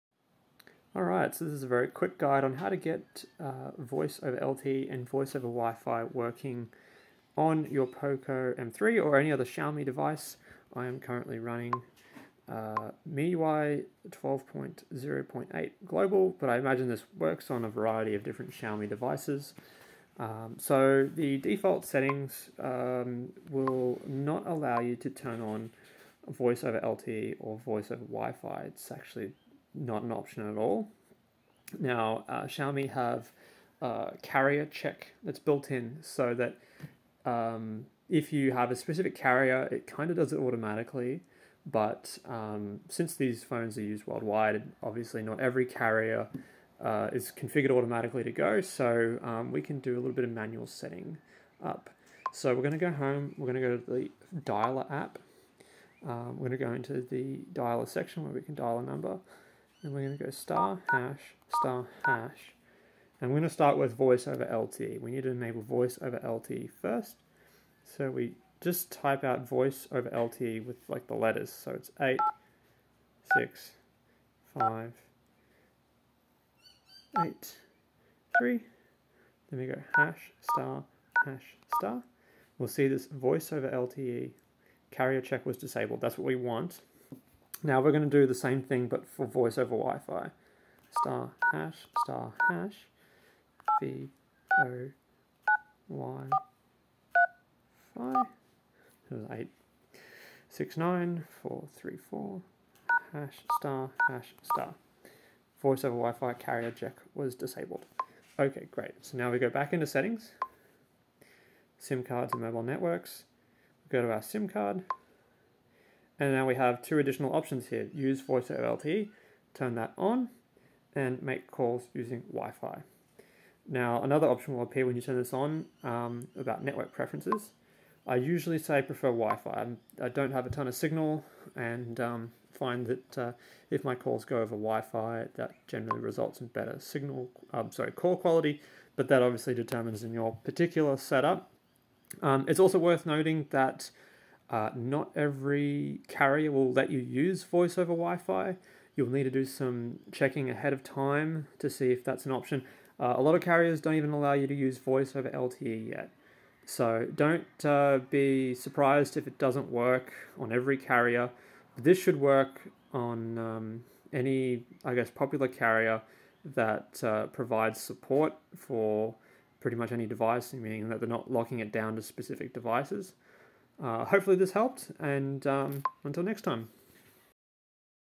Enable VoLTE & VoWiFi on your Xiaomi / Redmi / Poco on MIUI 12 (Carrier Check Disable Feature)
This is a quick video on how to enable Voice over LTE and Voice over Wi-Fi for your Xiaomi, Redmi or Pocophone device. For quick reference - the dial codes you need to enter to disable carrier check are the following:

VoLTE =  * # * # 86583 # * # *
VoWiFi =  * # * # 869434 # * # *

This is recorded on a Poco M3 running MIUI 12.0.8 Global. Please make sure your carrier supports these features for best results. Your ROM will also need the configuration files to support your carrier for Voice over Wi-Fi - MIUI 12 does not contain all the necessary files for all carriers on all devices. If this is your situation, you may need to root your device and install a custom ROM, or wait for Xiaomi to include these for your specific device (which may never happen, knowing Xiaomi).